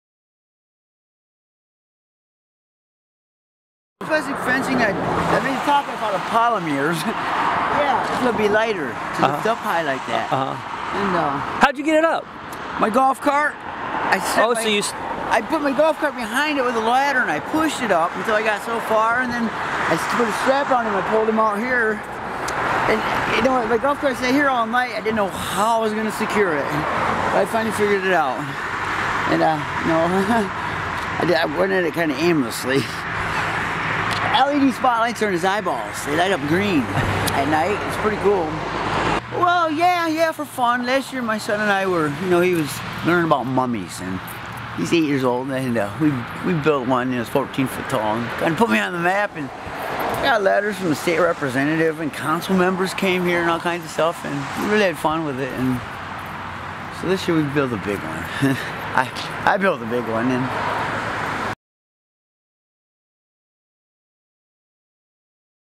Richard Underly talks about his giant mummy
Richard Underly&apos;s giant mummy at 9124 S. Westnedge in Portage just got bigger. At 23 feet high, the mummy is reaching nearly twice the size as last year’s Halloween decoration.